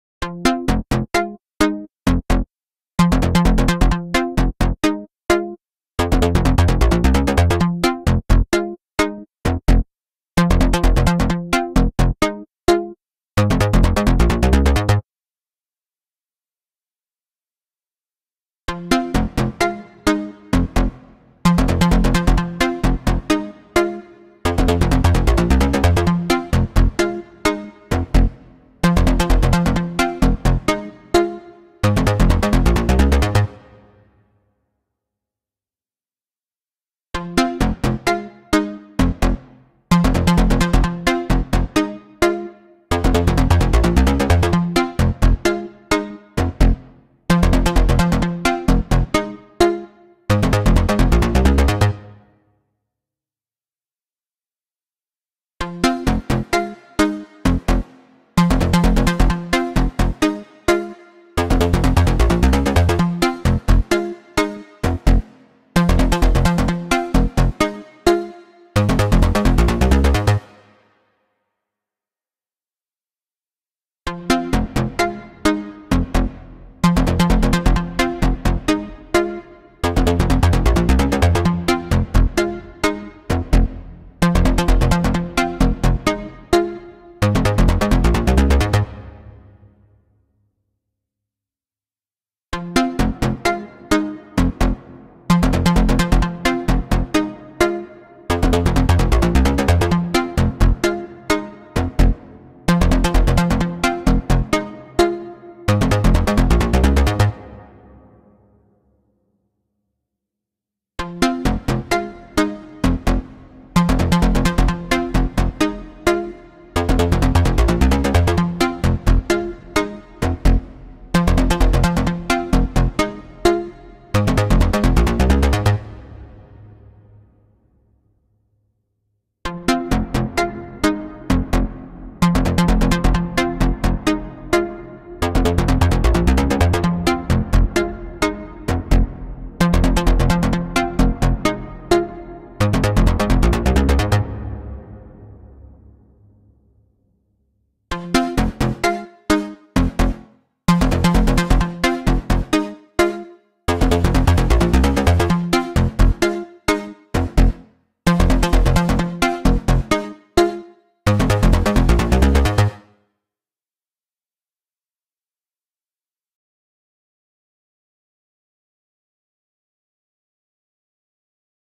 R2 - Plucks
Synth pizzicato-like sounds. Thanks so much to Sami Rabia of Aiyn Zahev Sounds for these examples.